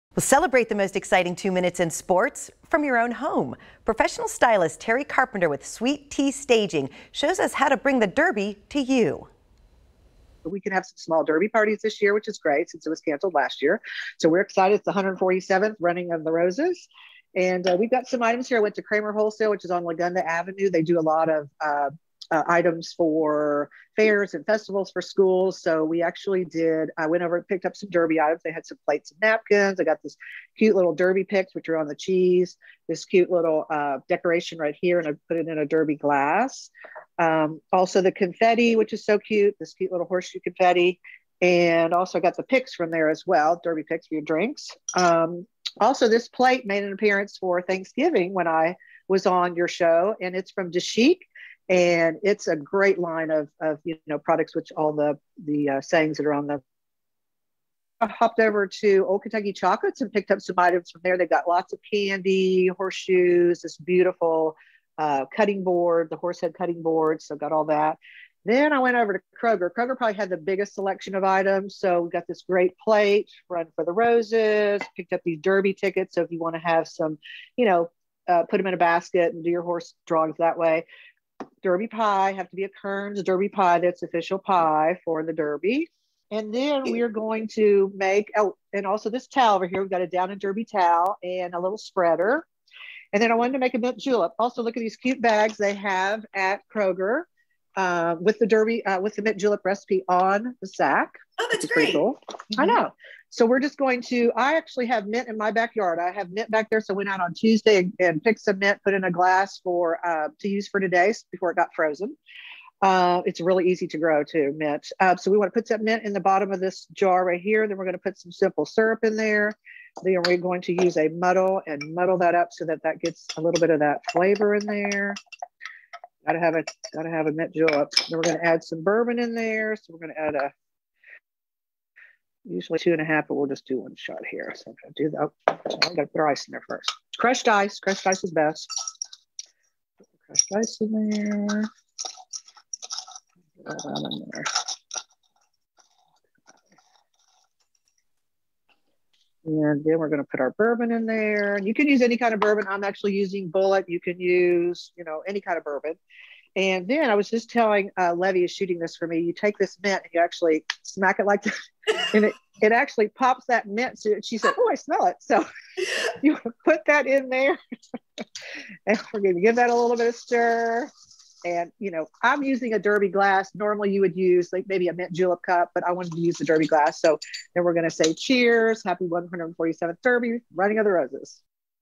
Sweet 'T' Staging has ideas on how to host your own Derby bash
The Kentucky Derby is back, and you can celebrate in style at home. Terri Carpenter, the owner of Sweet T Staging, has inexpensive ideas to make your Derby party a winner. You can get more ideas at sweettstaging.com.